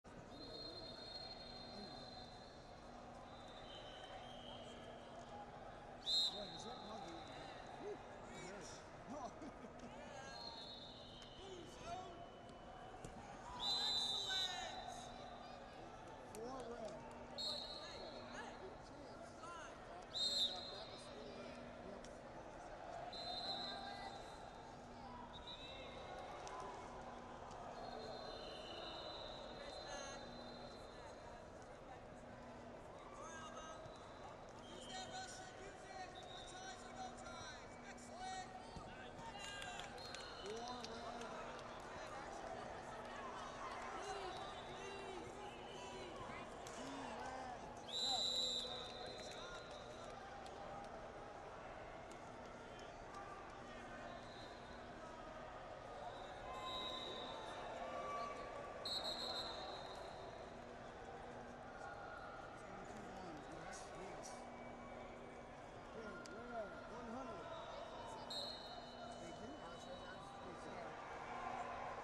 94 Lbs Placement Matches (16 Team) - Noah Rankin, Virginia Vs Alex Cook, Louisiana A287
2022 16U (Cadet) National Duals - 94 Lbs Placement Matches (16 Team) - Noah Rankin, Virginia Vs Alex Cook, Louisiana A287